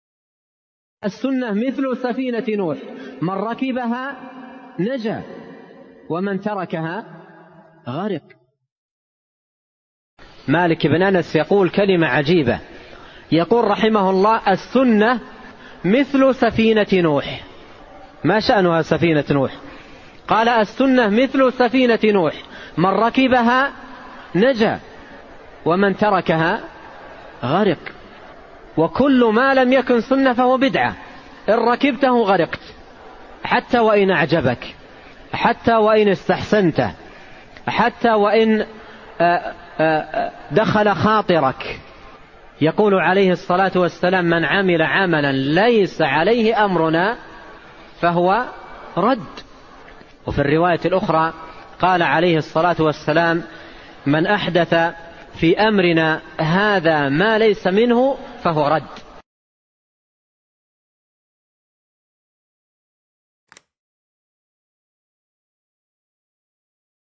The Sunnah is Like Nuh's Ark! | Shaykh Abdurrazzaq al-Badr
Shaykh ʿAbd al-Razzāq al-Badr حفظه الله speaks about the statement of Imām Mālik in which he likened the Sunnah to Nūḥ's ark.
السنة مثل سفينة نوح | الشيخ عبدالرزاق البدر حفظه الله